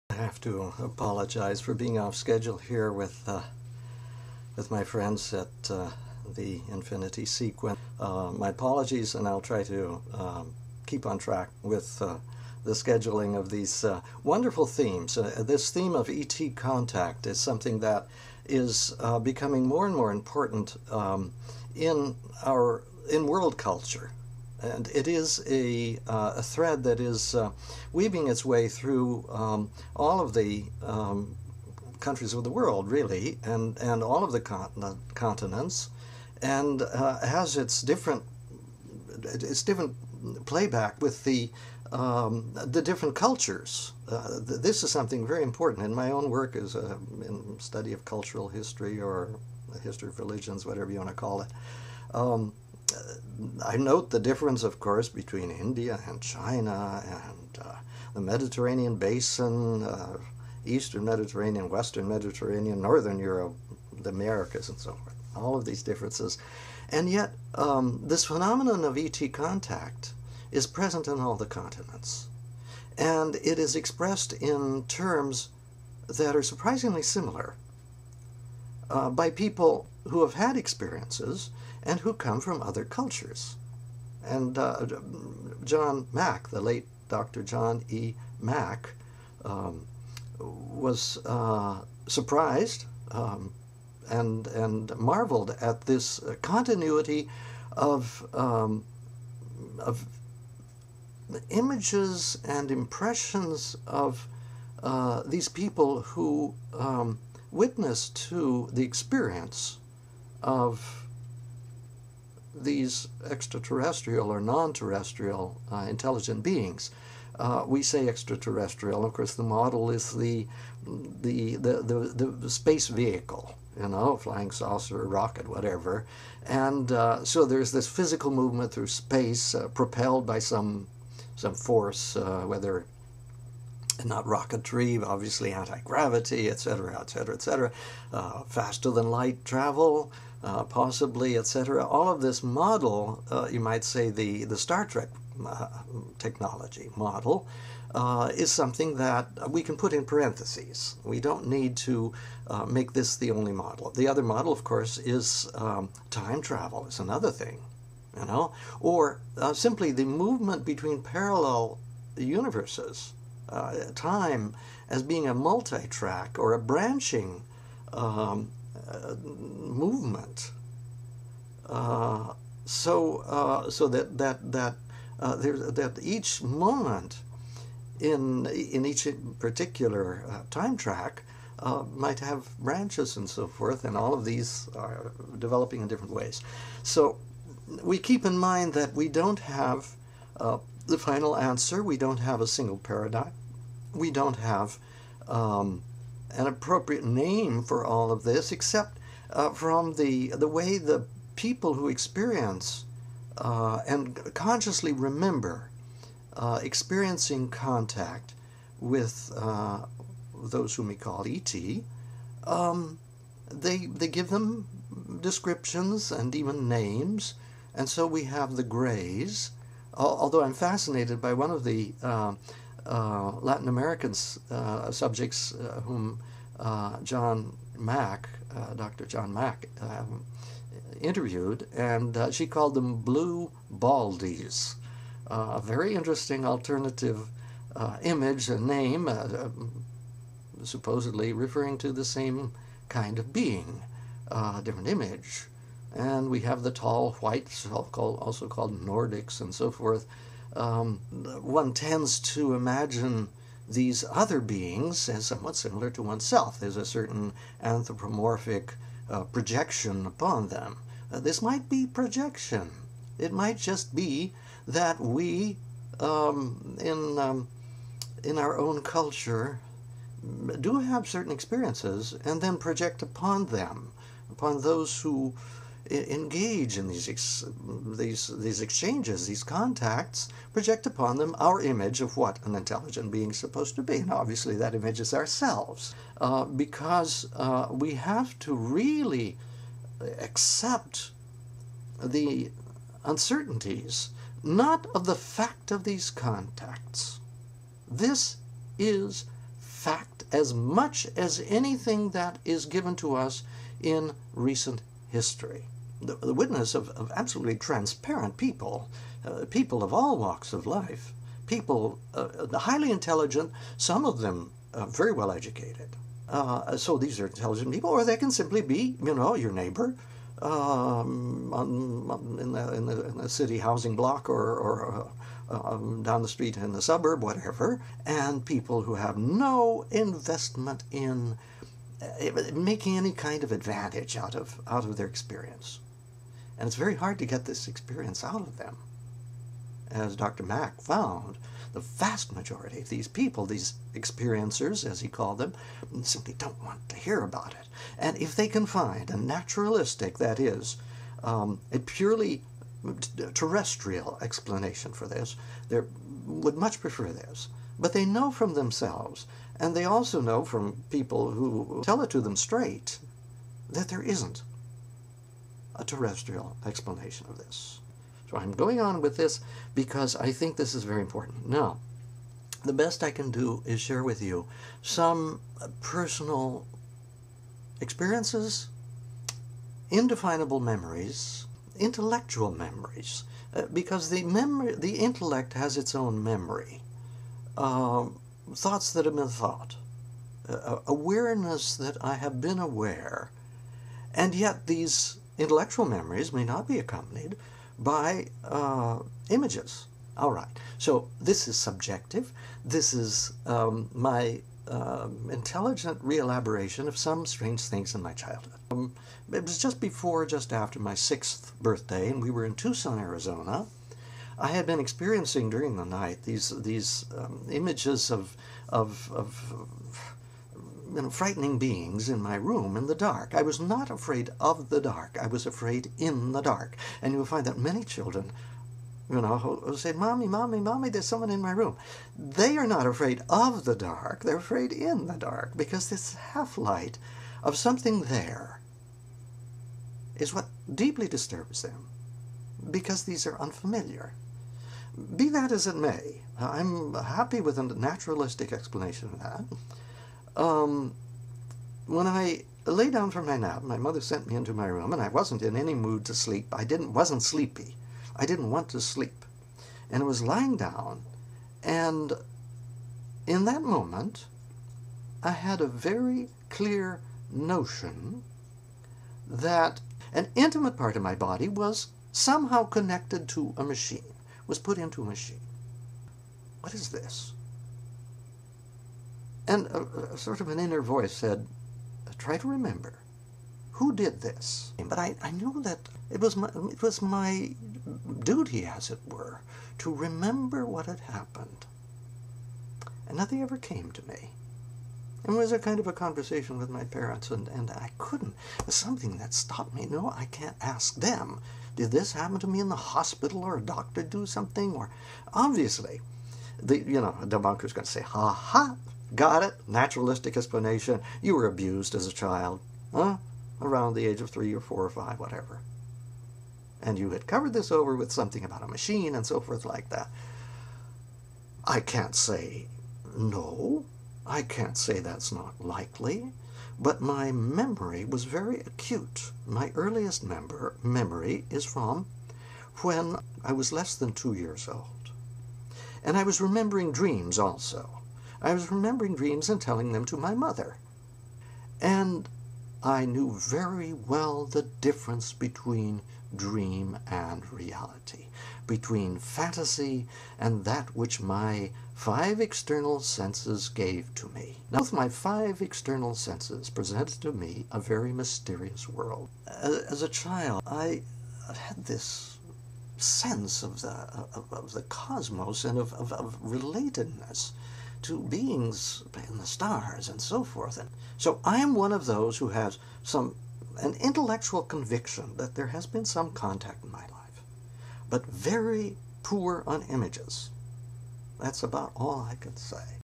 E.T. Contact
I am convinced, as a historian of culture, of the value of anecdotal evidence, including that of persons who, most of them quite reluctantly, narrate events of contact with non-terrestrial intelligent beings. I am much impressed by the studies that the late Dr. John E. Mack of Harvard made with hundreds of persons from several countries, whom he called "experiencers". Some spoke to him of the so-called "greys" (one person, from Latin America, called them "blue baldies"), those who abduct people, children and adults, and do various sorts of "medical" procedures on them.

Here I am sticking out my neck by recounting a childhood memory that could well be ascribed to a repressed episode of sexual abuse - were it so, and I knew this as a fact, I should feel rather comforted. As it was, this memory led me to an insatiable curiosity about the mysterious and very much non-terrestrial dimensions of the universe. I read about flying saucers in the newspapers in 1947, and I was convinced that they came from elsewhere than this planet. My reading in science fiction was not escapism but an unsatisfied search for a clear narrative about my own connection with the universe populated by intelligent beings.

I think that many people share my thoughts and experience on this. The anecdotal evidence they offer is no less robust than the material that a historian of culture must work with, to reconstruct the ways of thinking and living in ancient societies, such as India, China, or the Roman Empire.